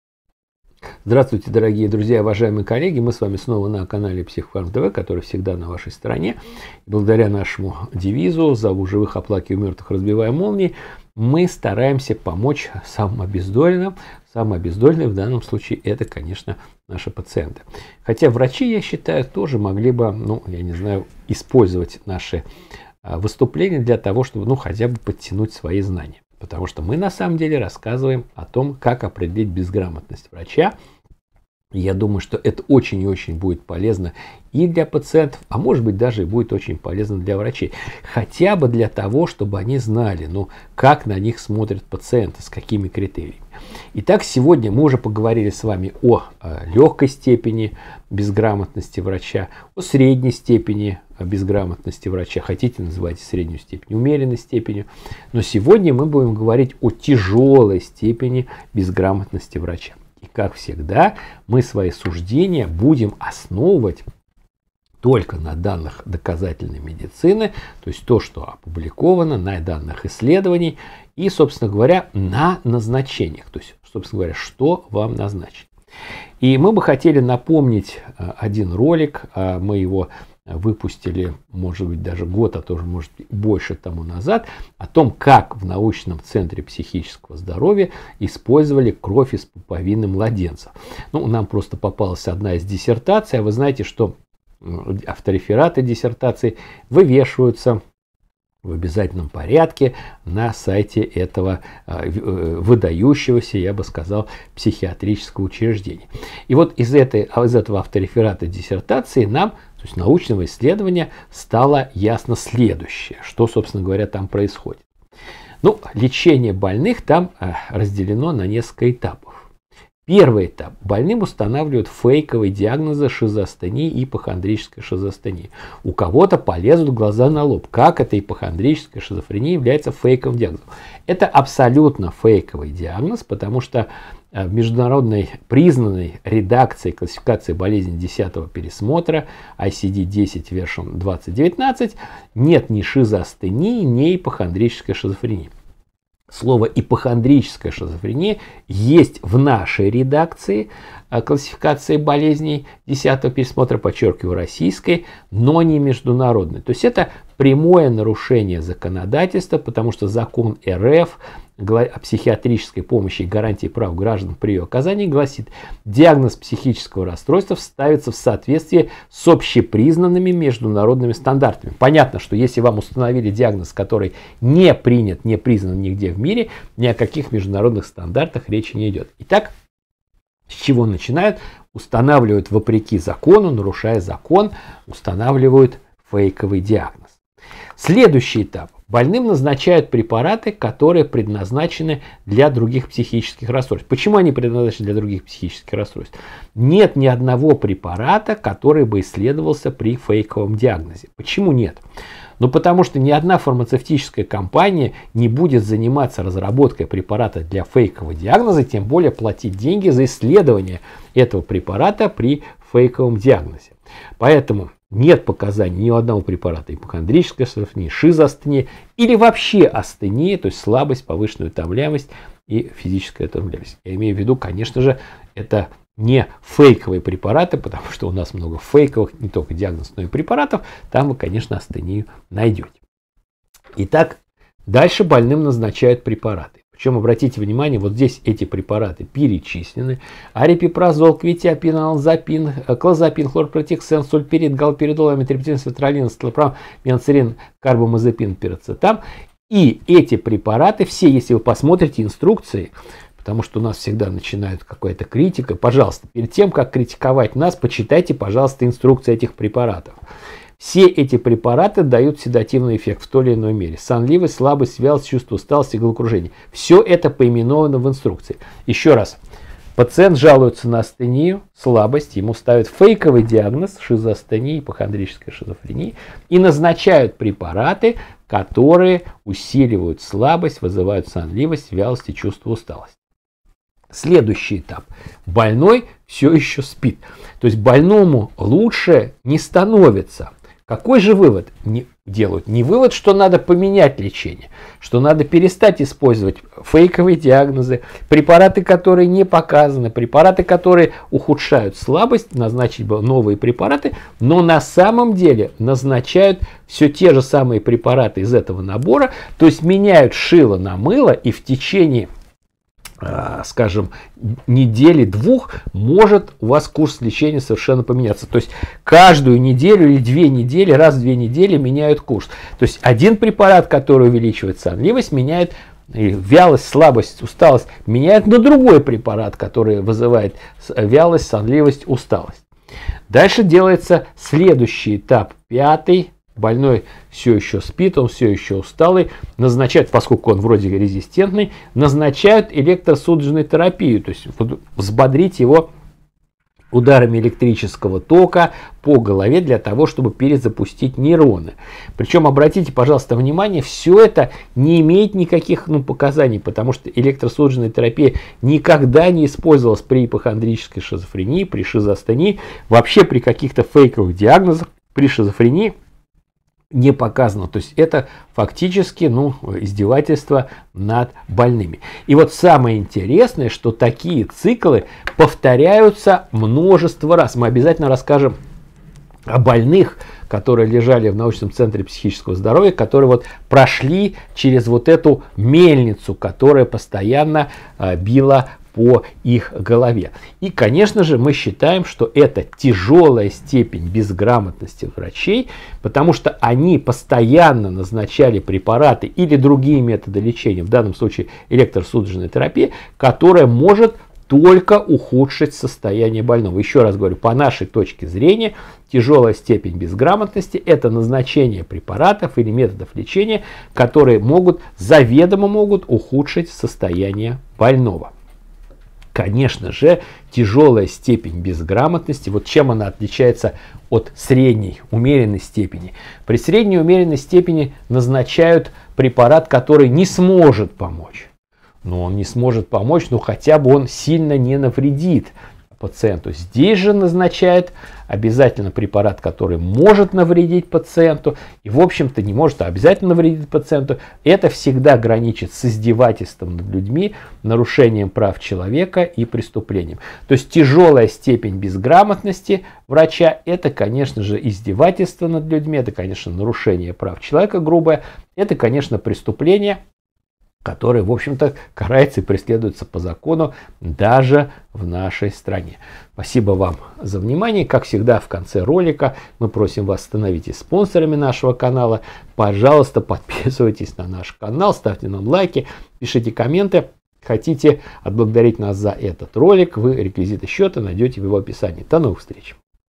Как определить тяжелую степень безграмотности врача?
Учим всех заинтересованных лиц определять тяжелую степень безграмотности врача по его назначениям. В качестве примера анализируем лечение астении в ФЕДЕРАЛЬНОМ ГОСУДАРСТВЕННОМ БЮДЖЕТНОМ НАУЧНОМ УЧРЕЖДЕНИИ НАУЧНЫЙ ЦЕНТР ПСИХИЧЕСКОГО ЗДОРОВЬЯ, который находится в Москве Вы будете шокированы!
Если Вам понравился наш ролик, Вы можете отблагодарить нас.
Как перевести нам деньги:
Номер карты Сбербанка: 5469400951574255